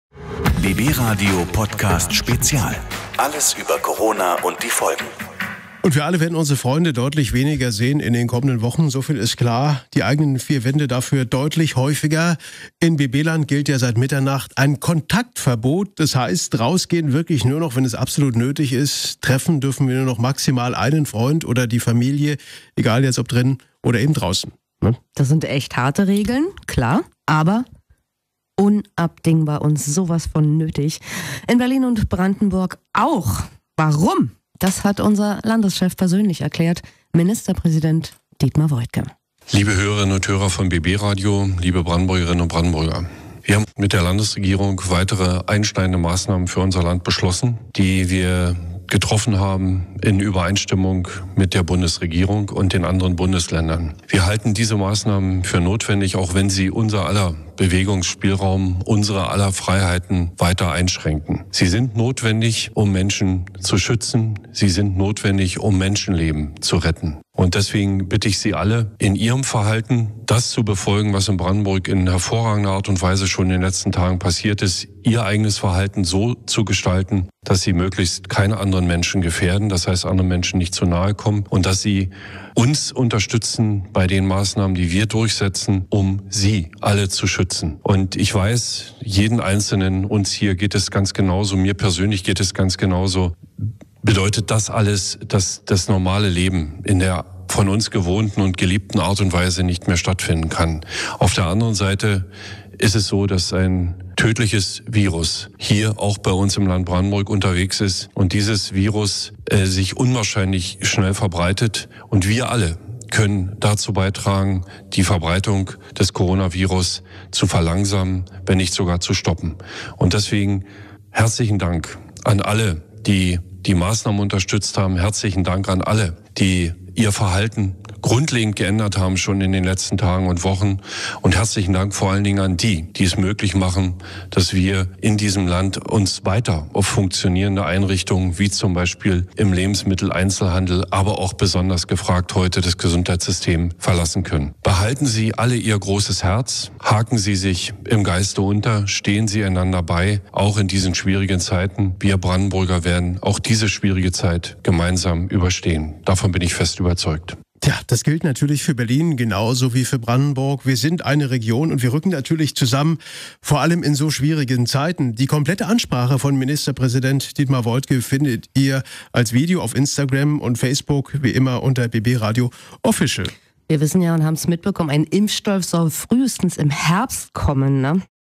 Corona - Ansprache Ministerpräsident Dr. Dietmar Woidke - BB RADIO Topthema Corona-Virus
Alles, was ihr zur Pandemie wissen müsst
Es vergeht kein Tag ohne neue Corona-Meldungen. Wir halten euch in der BB RADIO Morgenshow auf dem Laufenden und sprechen mit Experten über die Auswirkungen der Pandemie. Damit ihr immer bestens informiert seid!
Wir sprechen auch regelmäßig mit Brandenburgs Ministerpräsident Dr. Dietmar Woidke. Hier seine Nachricht zum Thema Kontaktverbot für alle Menschen in BB Land!